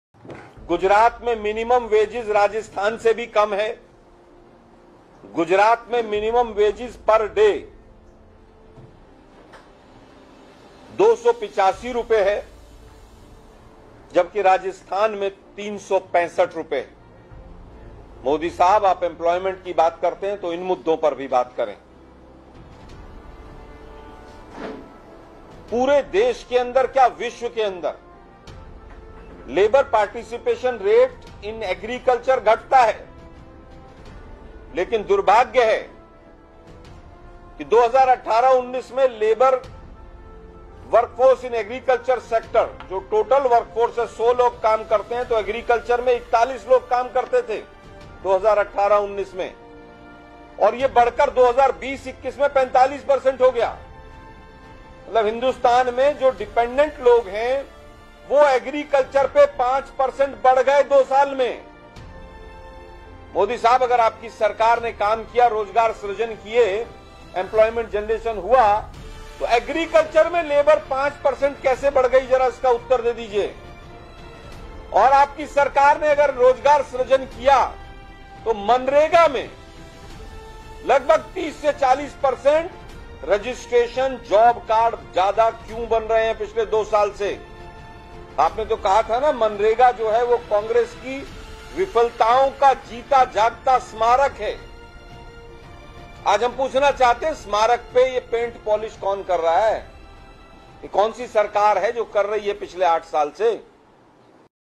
ગુજરાતની ભાજપ સરકાર પૂરતું લઘુત્તમ વેતન ના આપીને શ્રમિકોનું શોષણ કરી રહી છે.
ગુજરાતની ભાજપ સરકાર પૂરતું લઘુત્તમ વેતન ના આપીને શ્રમિકોનું શોષણ કરી રહી છે. : શ્રી Alok Sharma જી